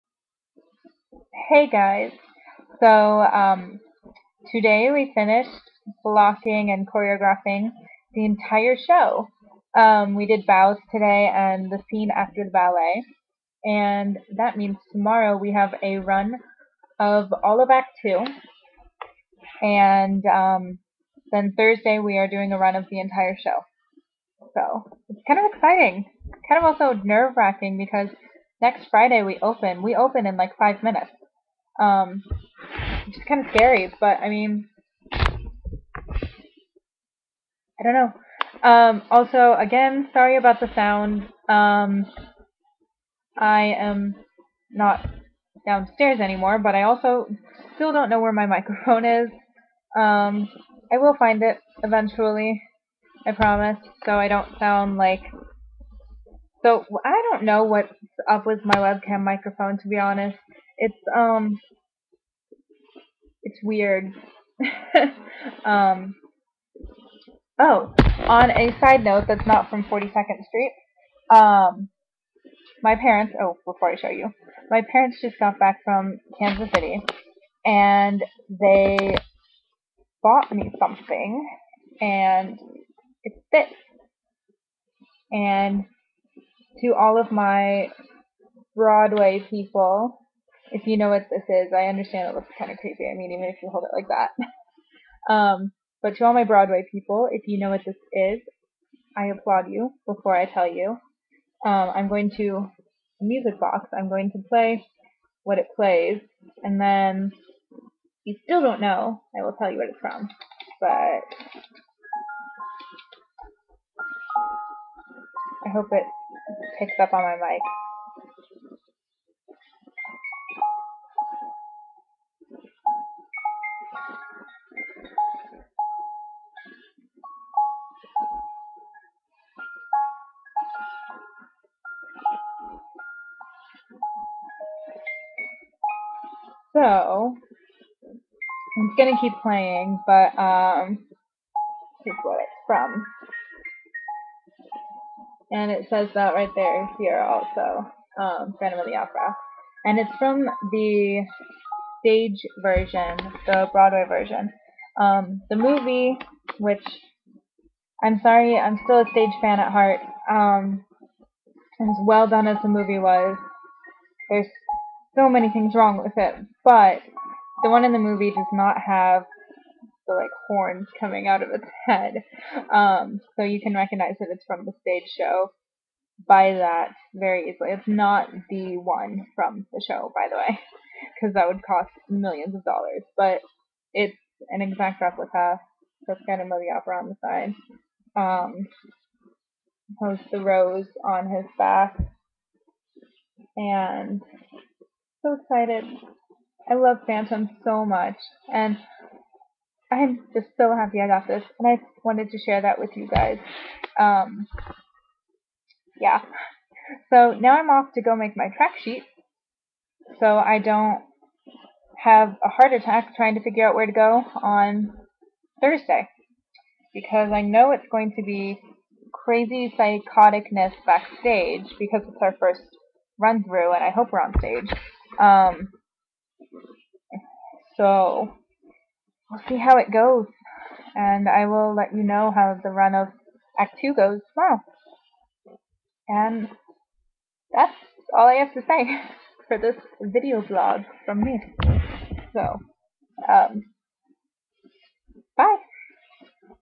VLOG # 4 42ND Street - Naples Players Teen Cast
Finished the show! Now onto tech week, also an exciting present my mom bought me while on her trip to Kansas.